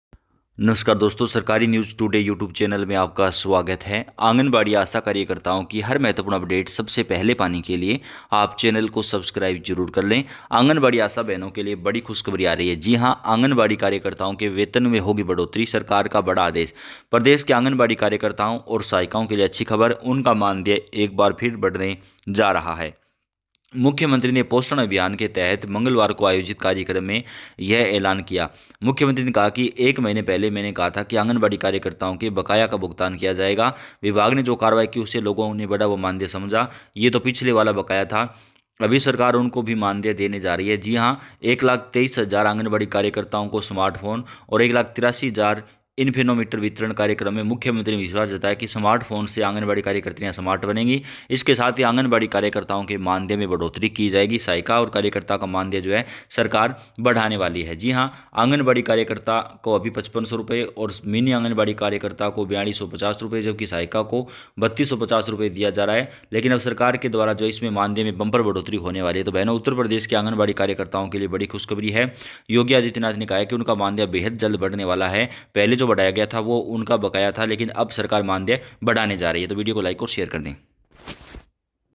आंगनबाड़ी कार्यकर्ता सहायिका खुशखबरी! मोदी ने लिखा पत्र वेतन बढेगा,Anganwadi
asha anganwadi in hindi
asha in anganwadi
anganwadi and asha workers
anganwadi and asha yojana honorarium